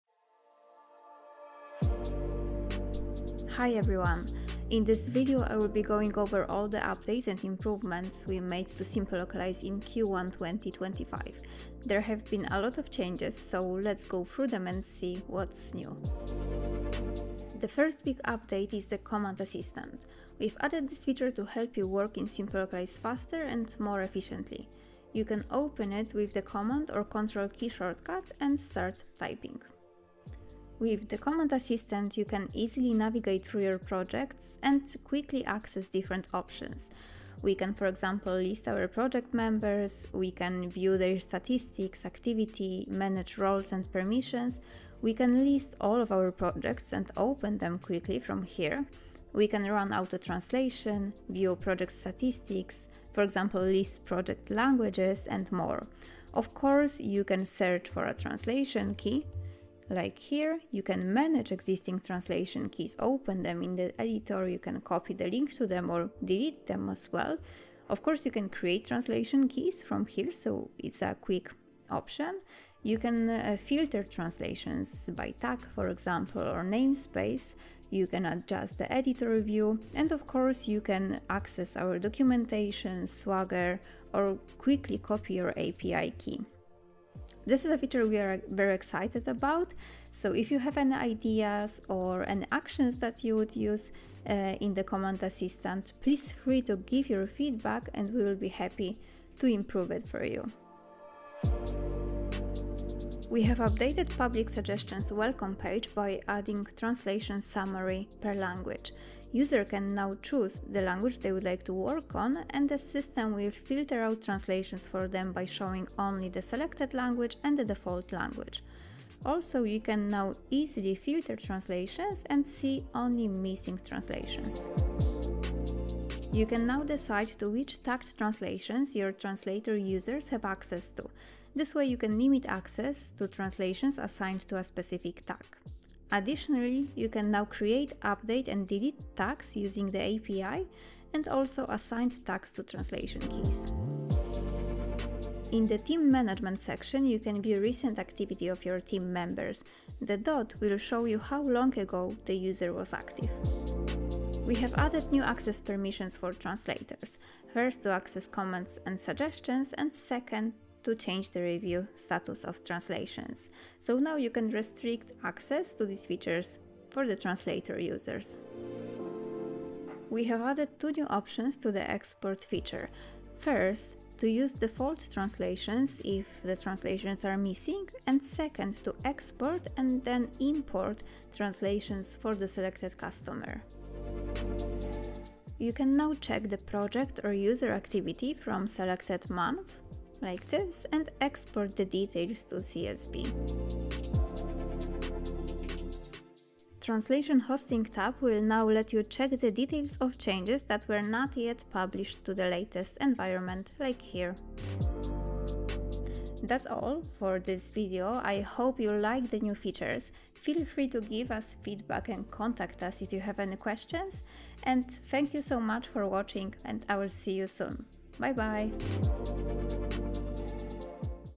What's new in SimpleLocalize - Q1 2025
Welcome to our Q1 2025 update! Join us as we dive into the latest developments, milestones, and exciting news from the last quarter of the year.

0:00 Intro
0:18 Command assistant
1:40 Public suggestion updates
2:10 Tag permissions
2:26 Last activity indicator
2:37 Access permissions
2:50 New export options
3:08 Activity updates
3:17 Pending changes
3:25 Outro